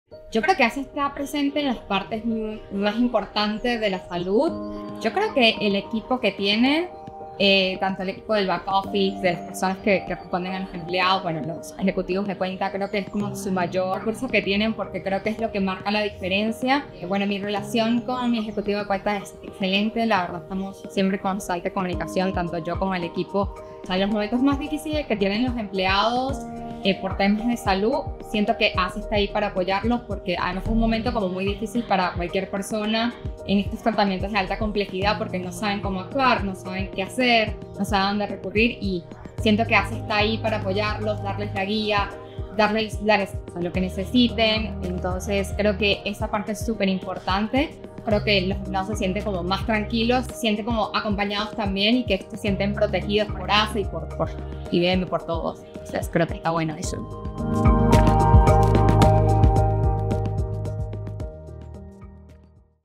ASE en primera persona: Maryory Arias de IBM
Charlamos sobre los aspectos esenciales de nuestro servicio y cómo es la experiencia de contar con ASE.

En ASE nos comprometemos con la salud y el bienestar de tu equipo a través de los mejores prestadores del país, con beneficios y planes personalizados.